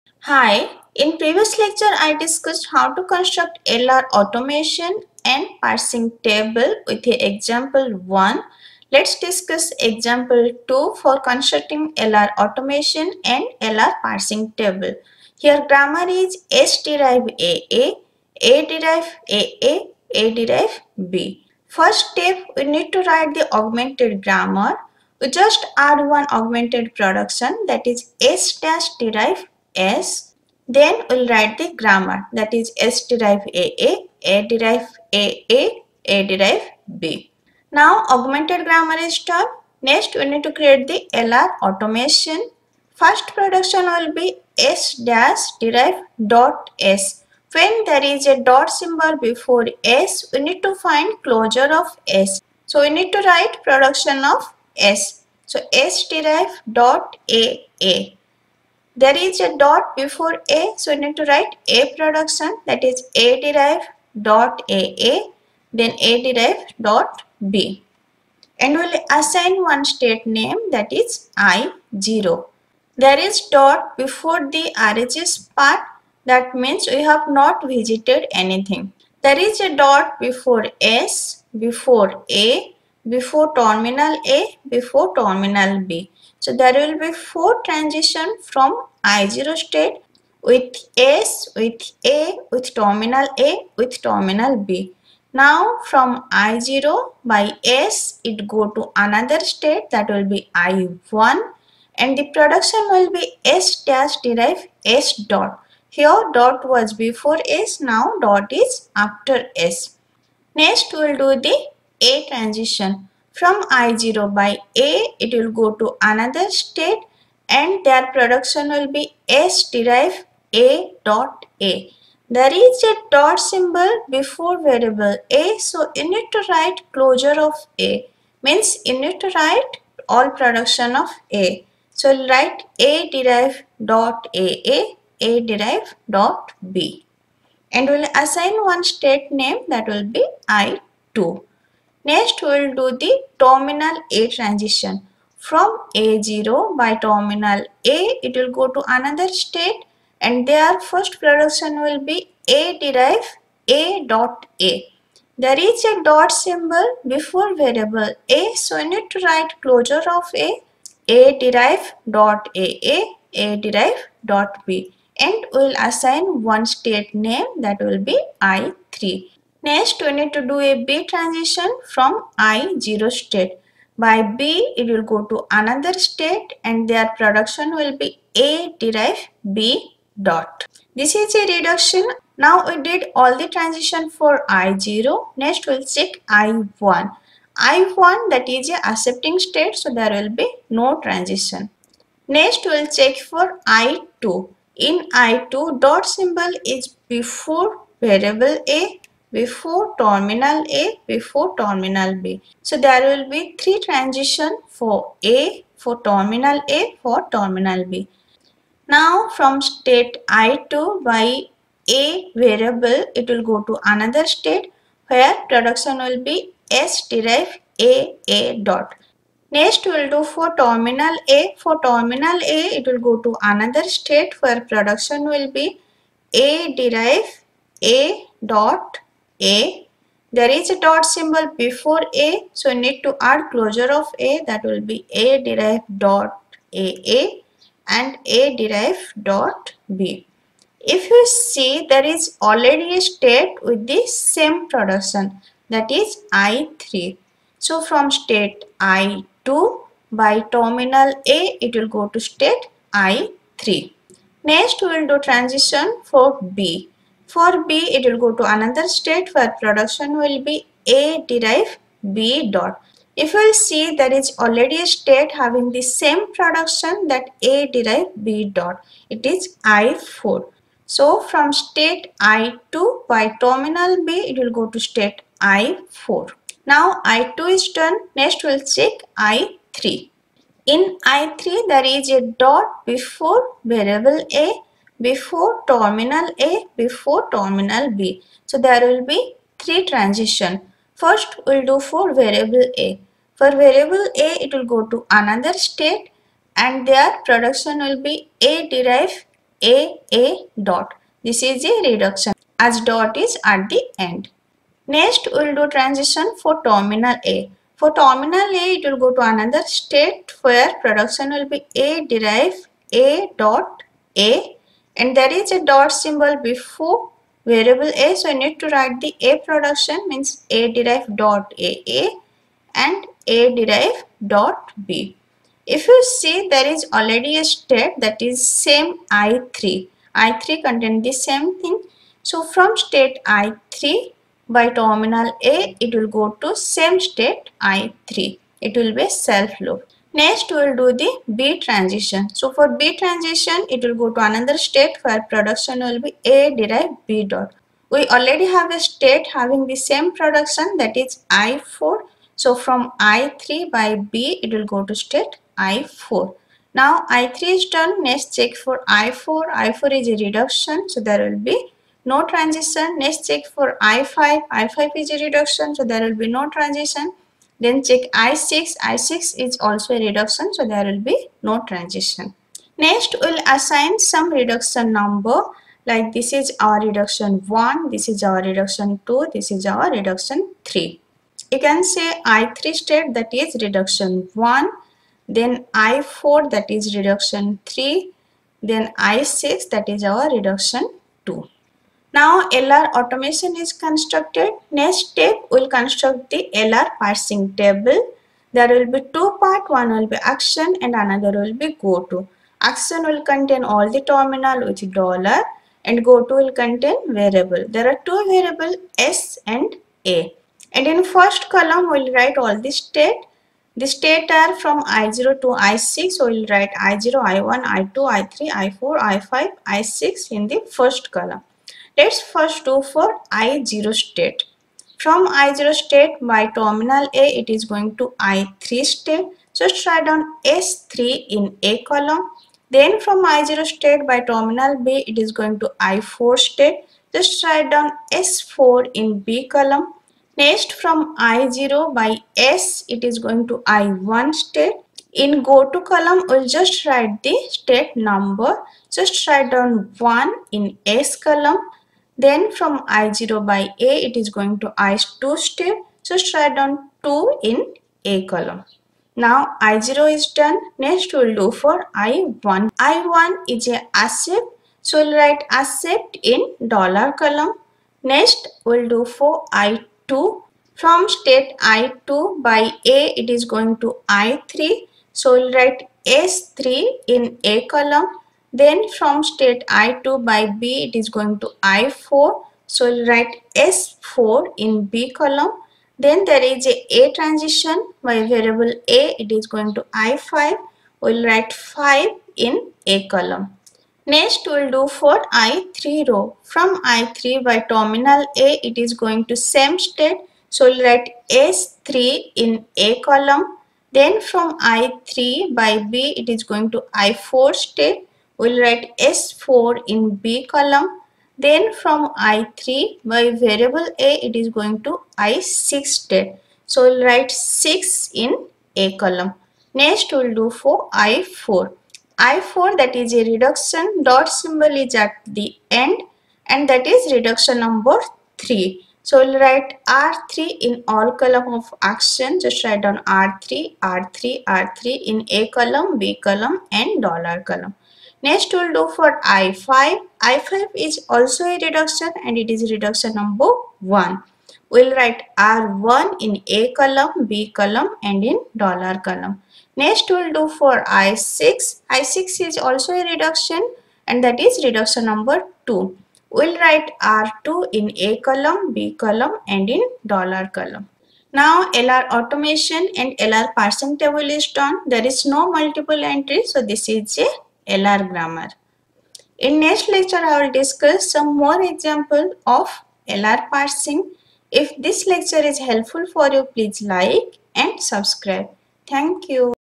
Ch 2.22:LR Parsing Table Construction with Ex 2: S →AA ,A →aA |b | CD Lectures for GATE
In this lecture i discussed Construction  of LR Automation & LR Parsing Table with
Ex 2: S →AA
          A →aA
          A →b